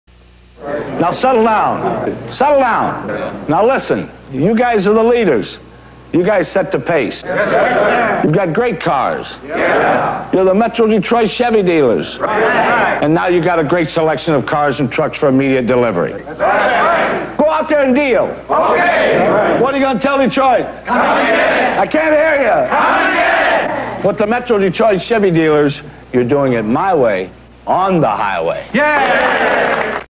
Sparky Anderson Chevy Dealers Commercial
Metro Detroit Chevy Dealers  commercial featuring Detroit Tigers Manager Sparky Anderson giving the team a pep talk.